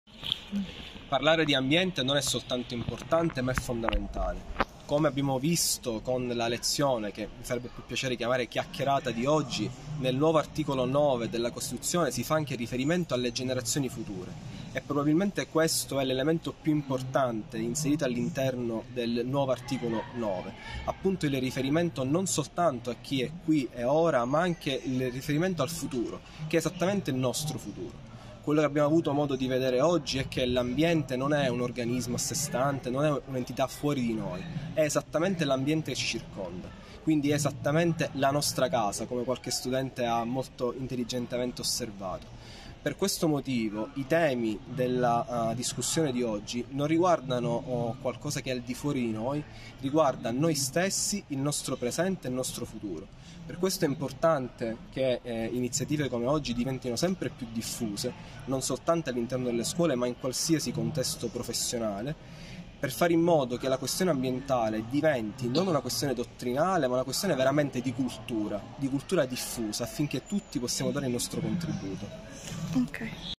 Marco Pasquale Marchese
avv. Marco Pasquale Marchese, esperto in diritto ambientale, il 16 dicembre 2022 ha tenuto una conferenza sulla riforma costituzionale in materia di ambiente.
Ha aperto l’incontro il Presidente del Consiglio dell’Ordine degli Avvocati di Palermo avv. Antonio Gabriele Armetta.
Intervista di Francy Dragotto